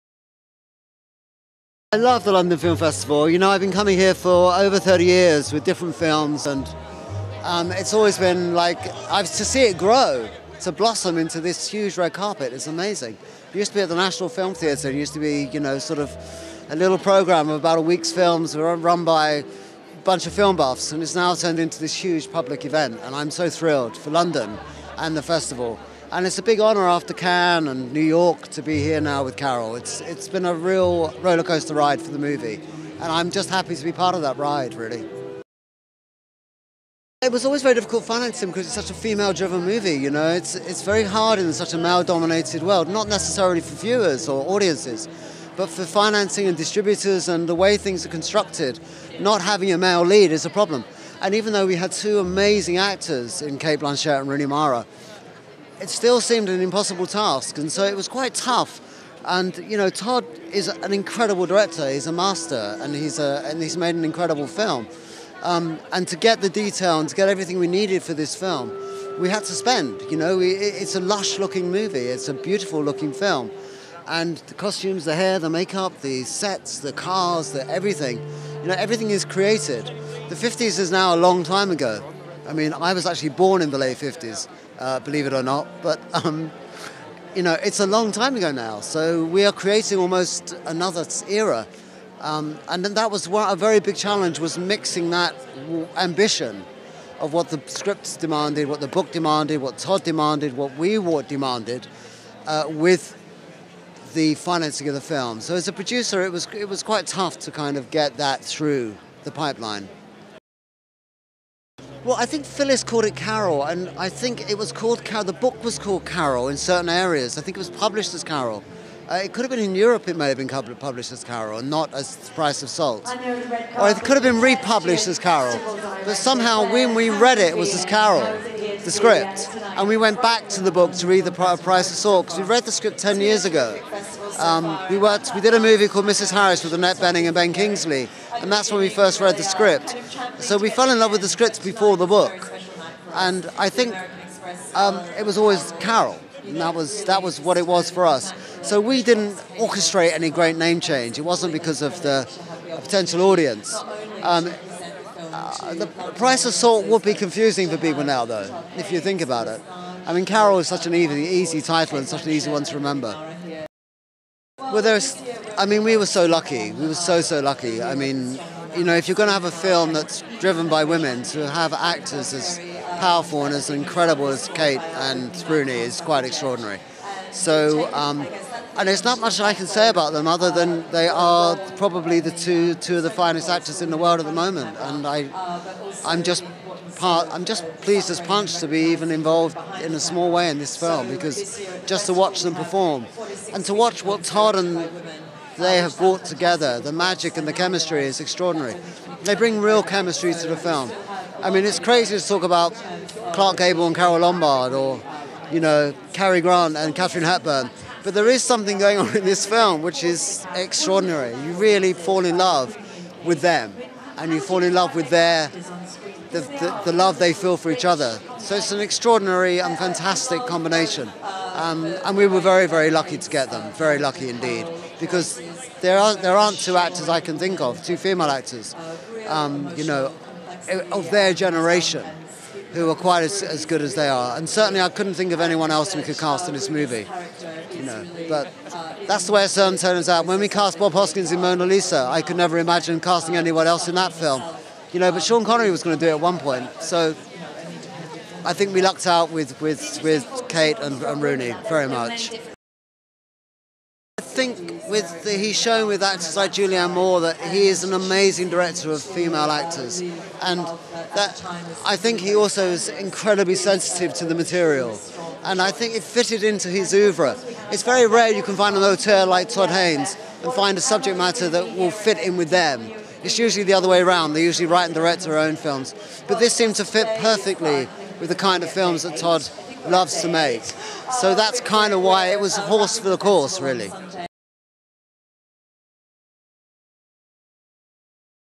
Carol Premiere Producer Stephen Woolley Interview
Red Carpet News talks to Cate Blanchett, Director Todd Haynes, Writer Phyllis Nagy, producers Elizabeth Karlsen & Stephen Woolley and Rooney Mara at the BFI London Film Festival premiere of LGBT romanti drama Carol.